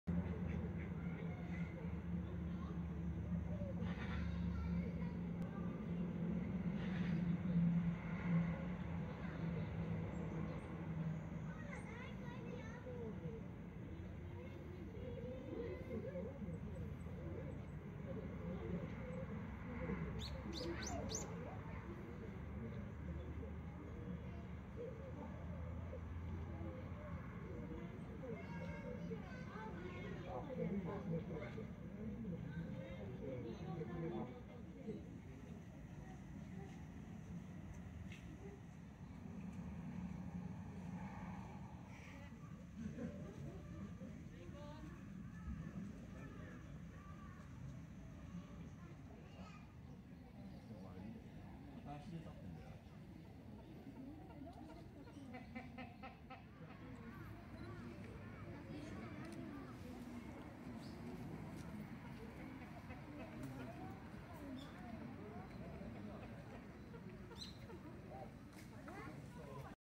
2022/01/23 ツキノワグマ 利子ちゃんのベッドメイキング
2022年1月23日撮影
豊橋総合動植物公園のんほいパークのツキノワグマの様子です

リコちゃんがお部屋に敷いてある落ち葉を手で掻き集めてフカフカのベッドを作っていました。